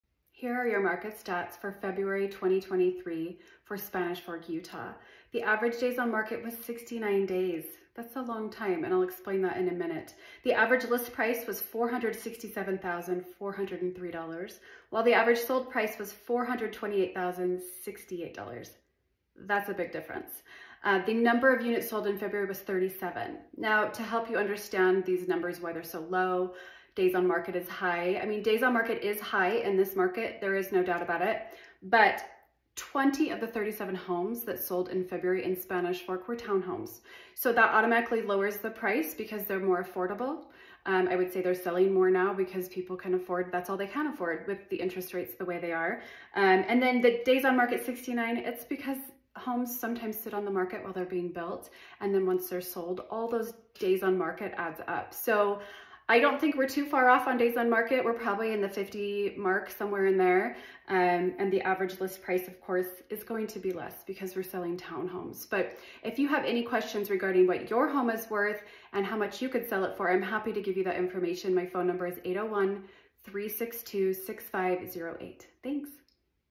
Market Report - February 2023 | South Utah real estate Market update
Market Report:
South Utah County Market Report. Days on market, average list price, average sold price and number of sales for the four major cities in south Utah county. All data was collected from utahrealestate.com